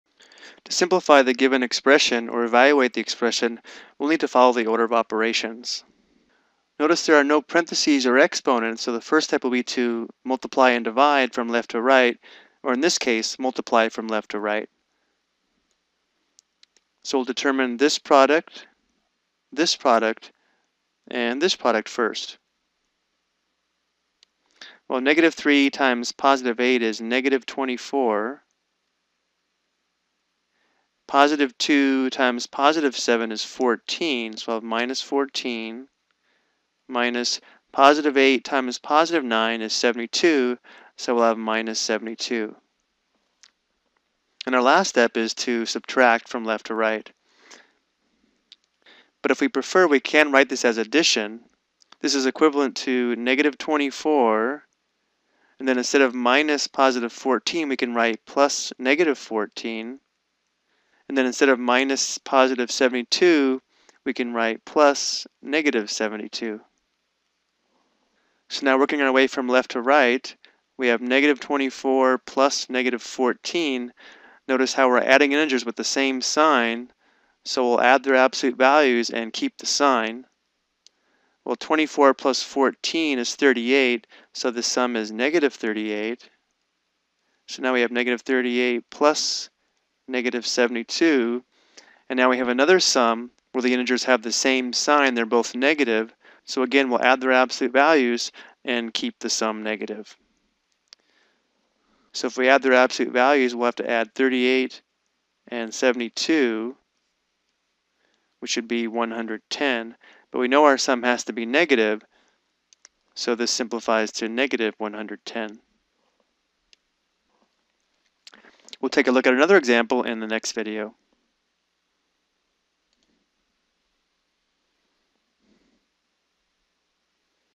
Ex 2: Simplify Expressions Involving Integers Using the Order of Operations
This provides an example of how to simplify or evaluate an expression involving integer operations using the order of operations.